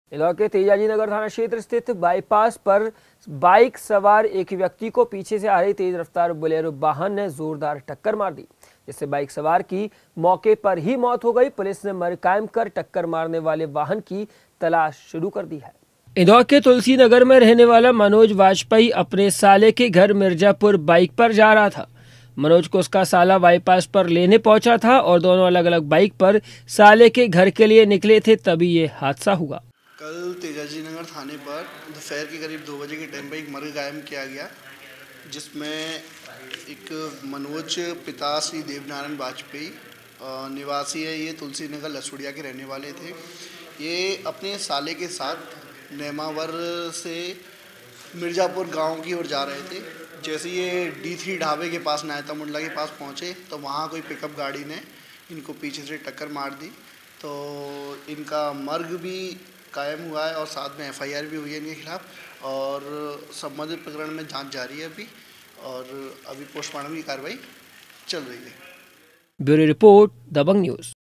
INDORE तेज रफ्तार बोलेरो ने बाइक को मारी टक्कर
इंदौर के तेजाजी नगर थाना क्षेत्र स्थित बायपास पर बाइक सवार एक व्यक्ति को पीछे से आ रही तेज रफ्तार बुलेरो वाहन ने जोरदार टक्कर मार दी । जिससे बाइक सवार की मौके पर ही मौत हो गई । पुलिस ने मर्ग कायम कर टक्कर मारने वाले वाहन की तलाश शुरू की है ।